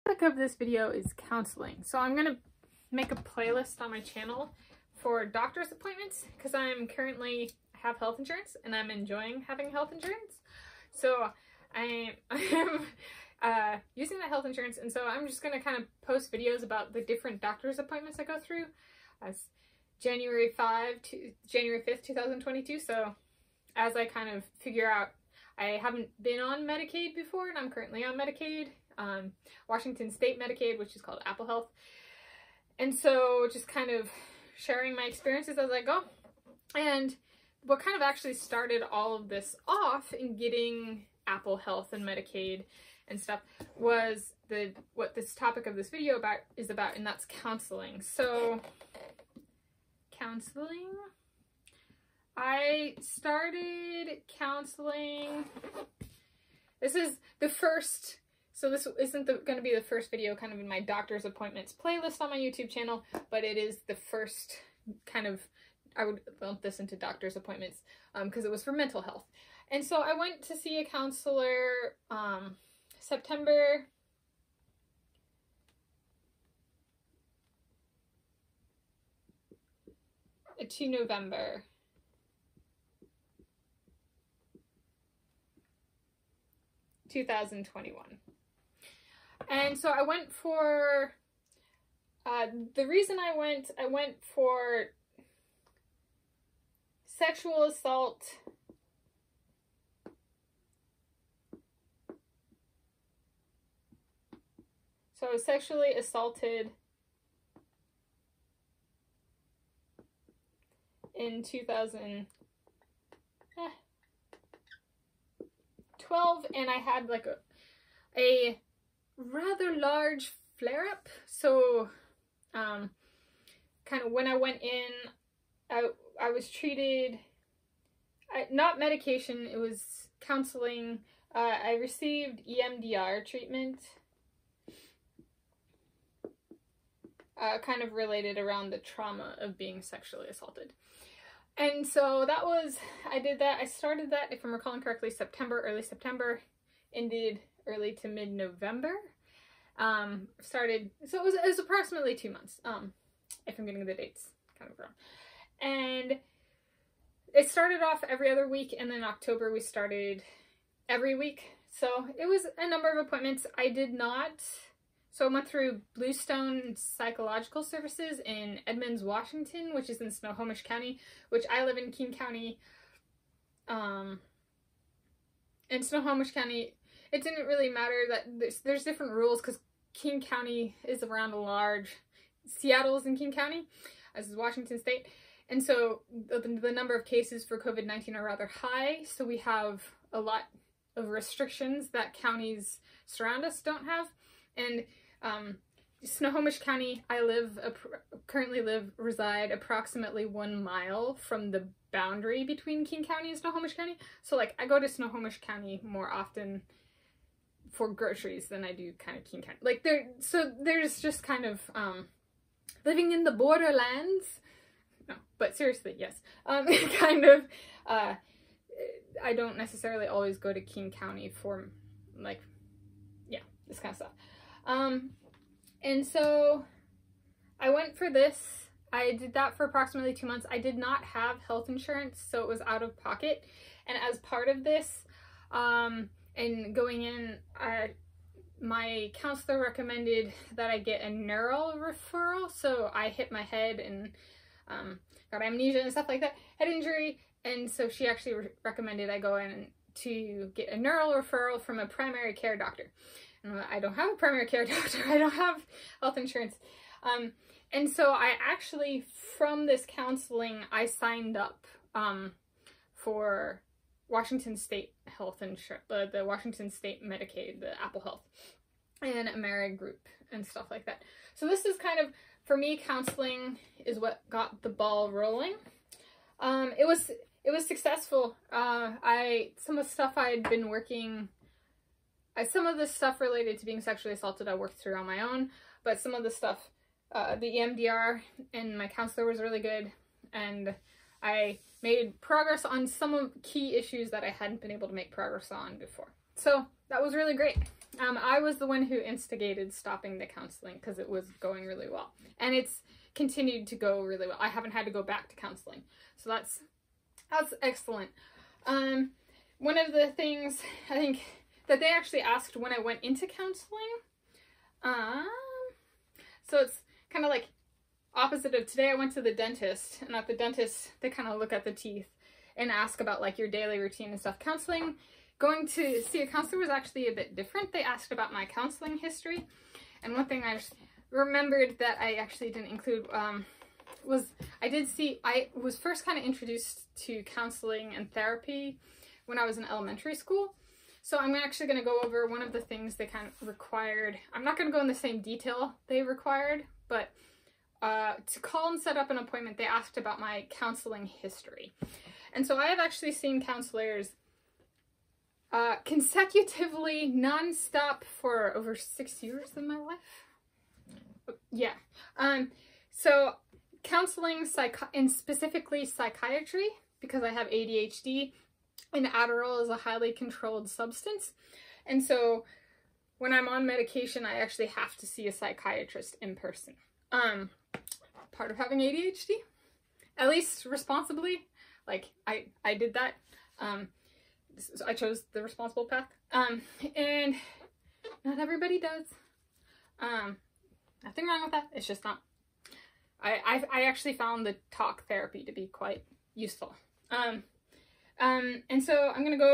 2021 experience in counseling and my counseling history - Part 1 of 2
I was in counseling from September to November 2021 for issues related to being sexually assaulted in 2012. In this Part 1 of 2, I cover my experience in those two months of counseling in 2021 as well as a brief overview of my history of receiving counseling prior to 2021.

Here is the link to Part 2 of 2, which covers my experiences in counseling prior to 2021 by age and reason I received counseling

0:00 - Intro;
0:52 - Experience in counseling September to November 2021 (29 years old) for issues related to being sexually assaulted in 2012;
5:10 - Overview of my history in counseling.

Filmed by me (Tanis Coralee Leonhardi) in Shoreline, Washington State, United States of America in one take (part 1 and 2 filmed in one take, split into two videos for uploading purposes) starting at 5:08 p.m. (Pacific time) on Wednesday 5 January 2022. Premiered 5 January 2022.